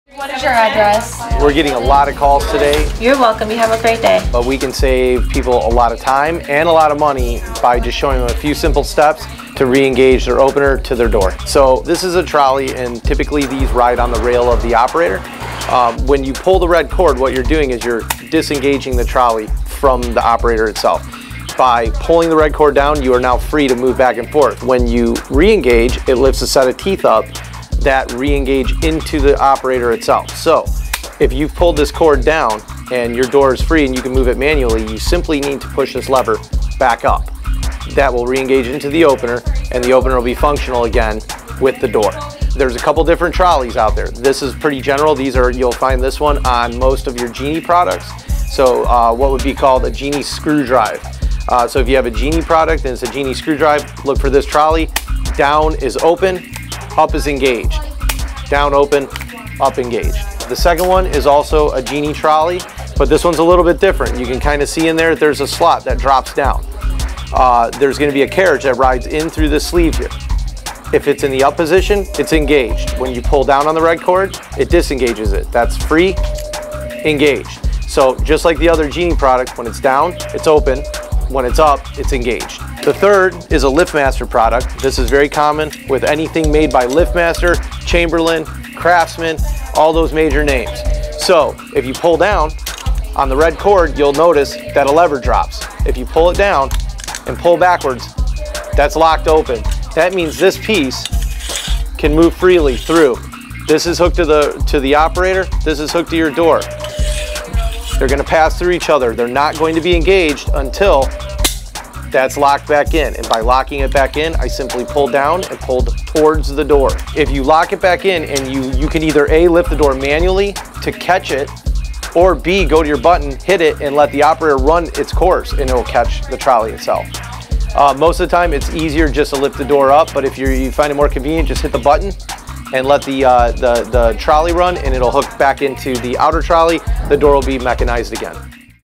Re Engage Garage Door Opener / Trolley
When your power goes out it's good to know how to diss-engage and re-engage your garage door opener.  We go through the different major brands and styles of trolleys you probably have in your garage.